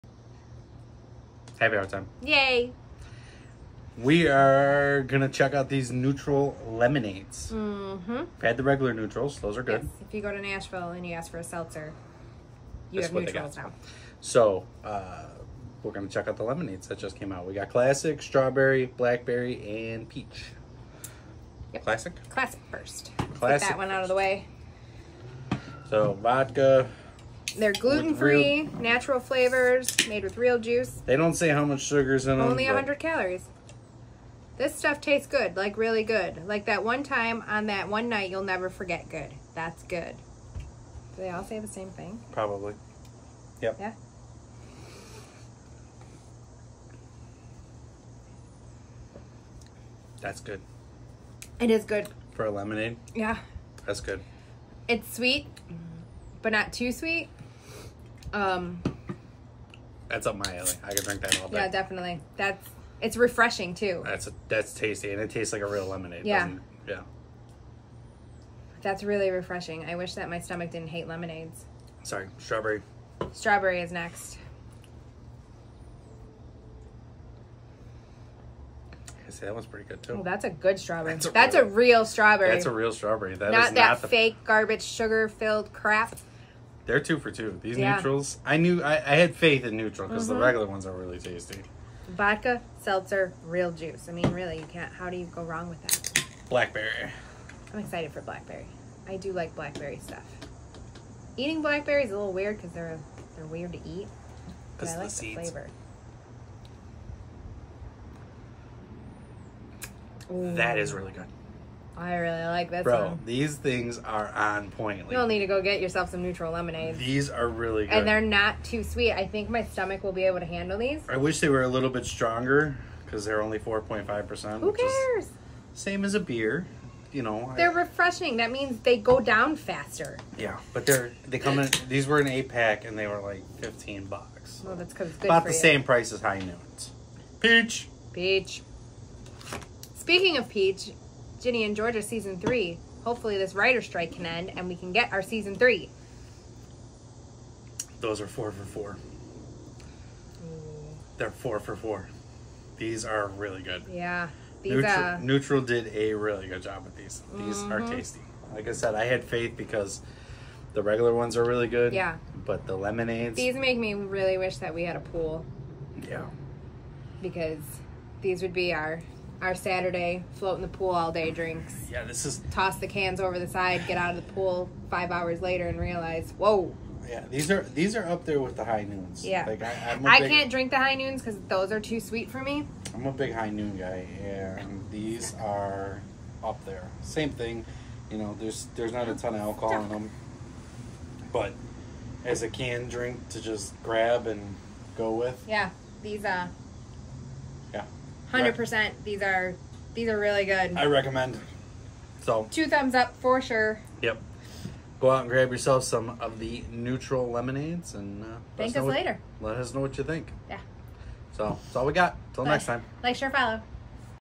Nütrl Lemonade review
Checking out the Nütrl Lemonades

These are fantastic!! They taste like real flavors not the artificial crap in a lot of the cannedcocktails